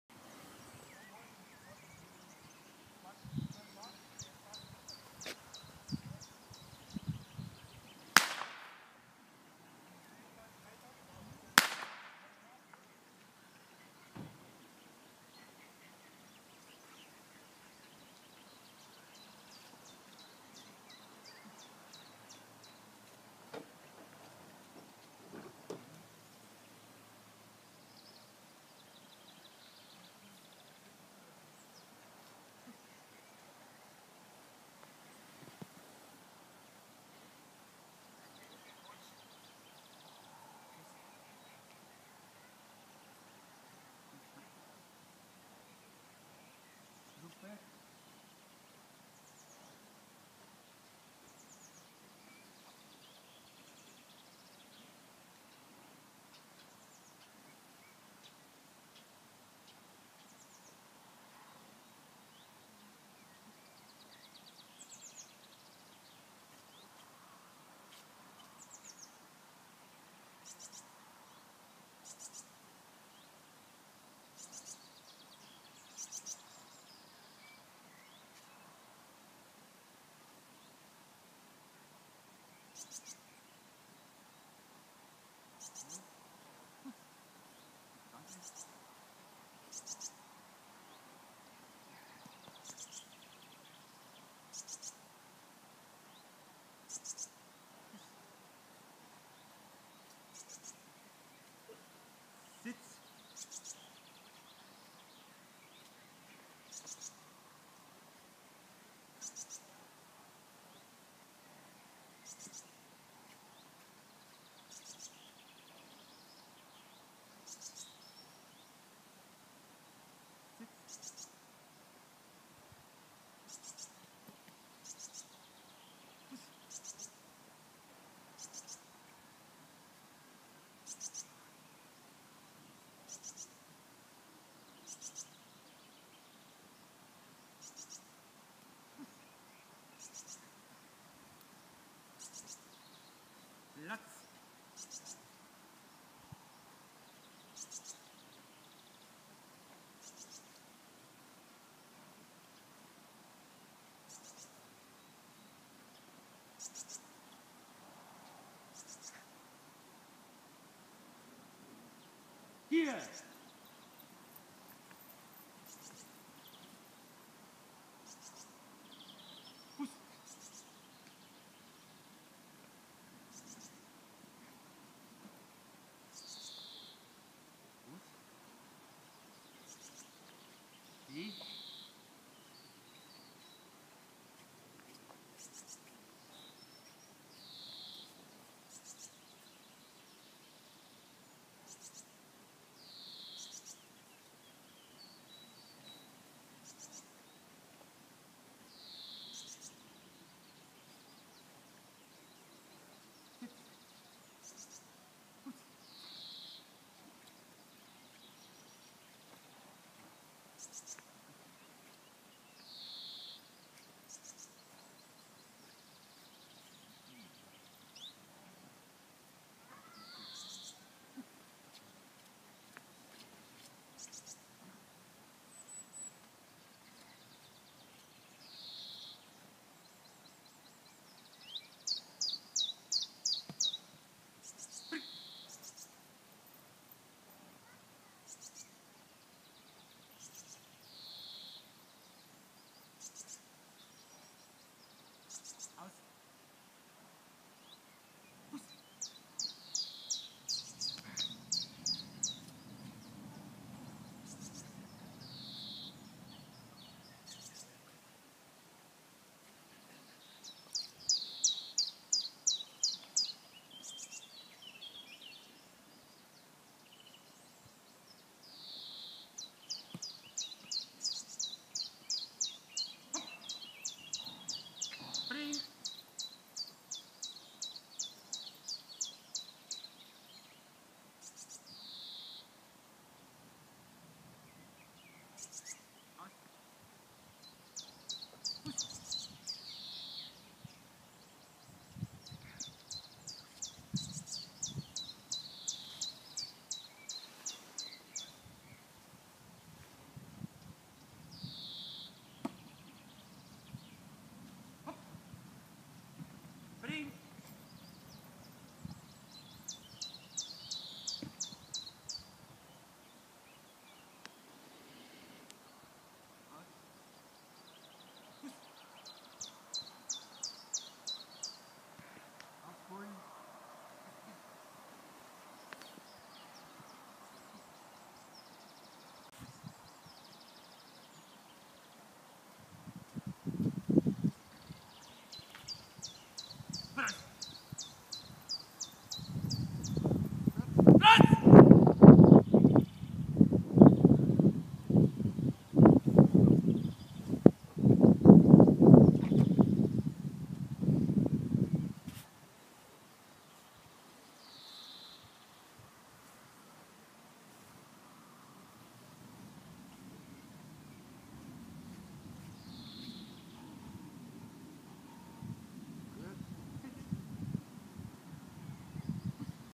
Bouvier des Ardennes Hasenhirsch Bazi IPO2 Abt.B - Unterordnung (2 Jahre)
GSV Wittelsbacher Land, Richter Johann Grübl